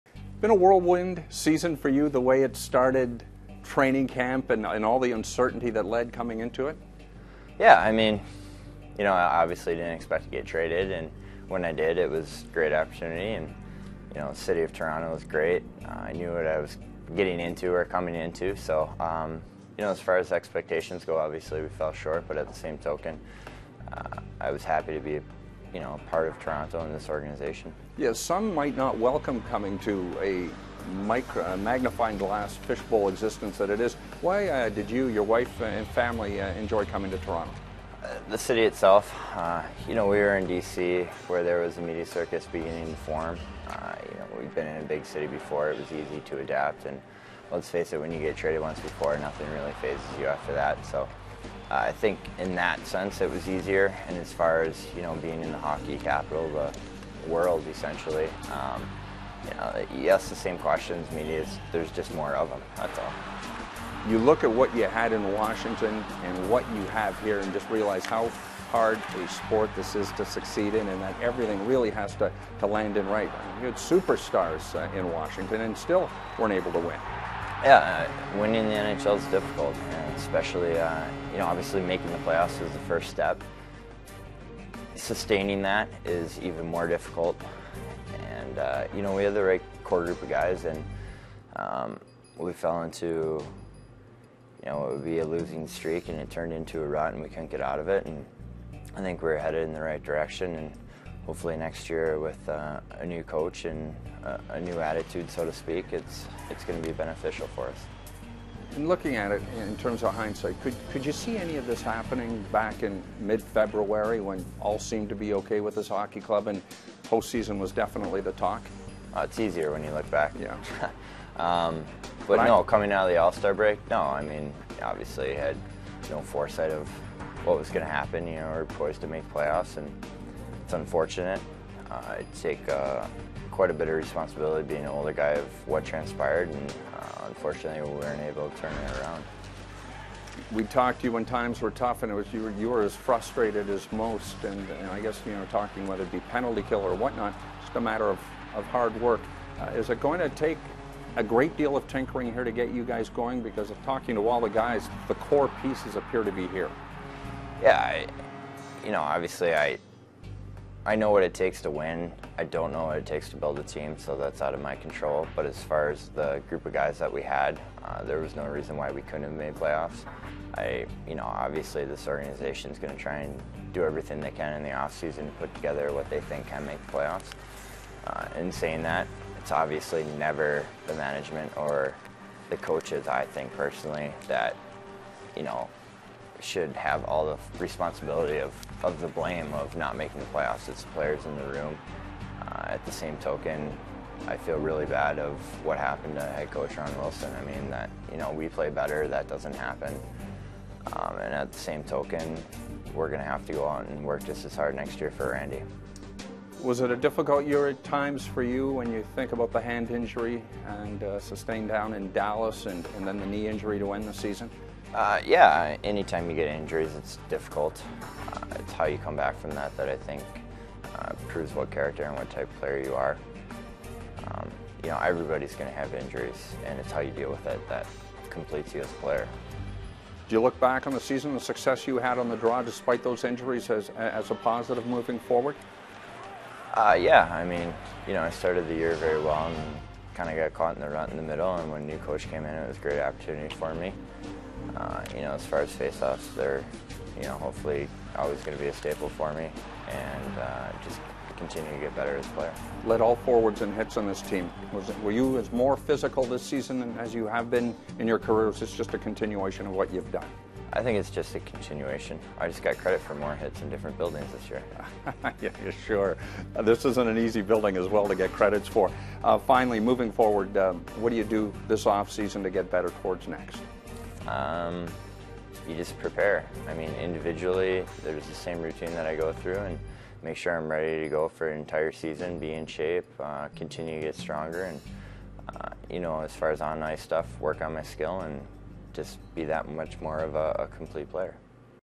Year End Sit Down: David Steckel
David Steckel talks about the team's highs and lows throughout the season, in an interview with Leafs TV's Paul Hendrick. For all the year end interviews, go to mapleleafs.com